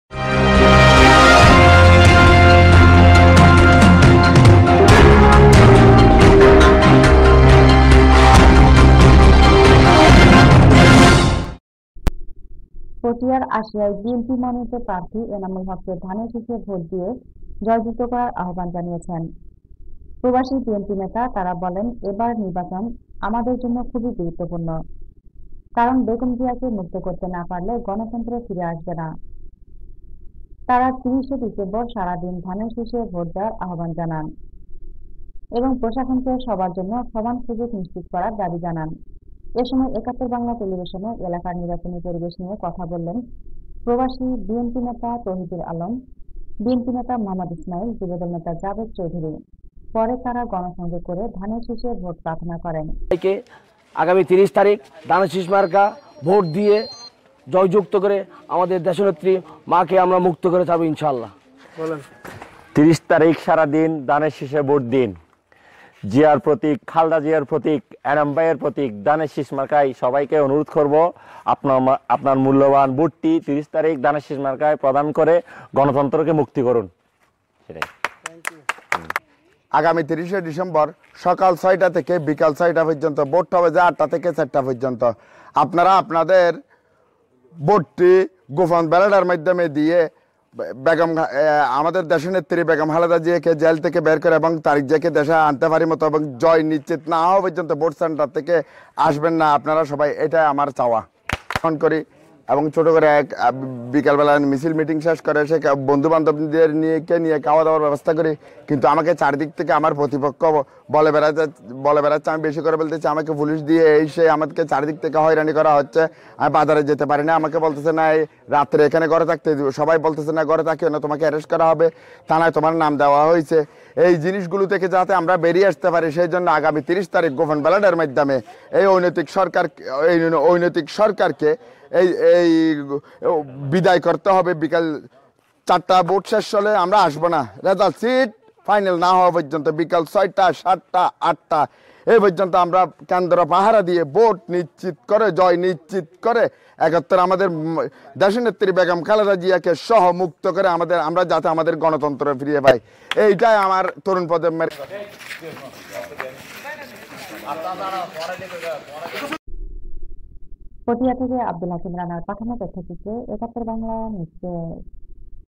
পটিয়ার আশিয়ায় ধানের শীর্ষকে বিজয় করার আহবান|Potia|71Bangla TV
পটিয়ার আশিয়ায় বিএনপি মনোনিত  প্রার্থী এনামুল হককে ধানের শীর্ষে ভোট দিয়ে জয়যুক্ত করার আহবান জানিয়েছে প্রবাসী বিএনপি নেতা তারা বলেন এবারের নির্বাচন আমাদের জন্য খুবই গুরুত্বপূর্ন। কারন বেগম জিয়াকে মুক্ত করতে না পারলে গনতন্ত্র
ফিরে আসবে না। তারা ৩০ ডিসেম্বর সারা দিন ধানের শীর্ষে ভোট দেওয়ার আহবান। এবং প্রশাসনকে সবার জন্য সমান সুযোগ নিশ্চিত্ব করার  দাবি জজানান।এ সময় একাত্তর বাংলা টেলিভিশনে এলাকার নিবার্চনী পরিবেশ নিয়ে কথা বলেন প্রবাসী বিএনপি নেতা তৌহিদুর আলম, বিএনপি নেতা মো: ঈসমাইল যুবদল নেতা জাবেদ চৌধুরী। পরে তারা গন সংযোগ করে ধানের শীষে   ভোট প্রার্থনা করেন।